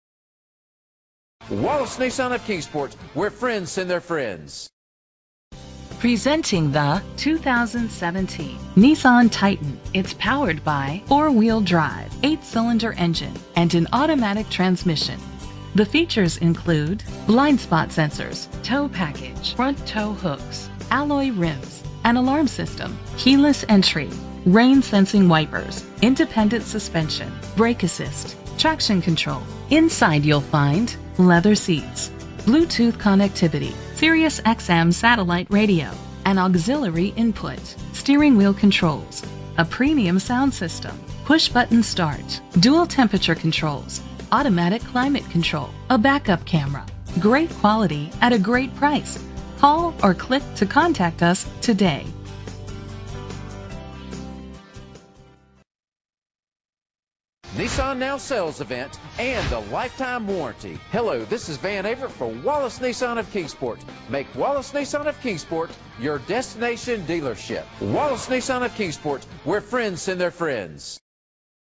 2017 Nissan Titan N3377 - Kingsport TN
Phone:
Year: 2017
Make: Nissan
Model: Titan
Trim: PRO-4X
Engine: 5.6 liter 8 Cylindercylinder 4WD
Transmission: 7-Speed Shiftable Automatic
Color: Black
Mileage: 1
Address:
VIN:1N6AA1E50HN526703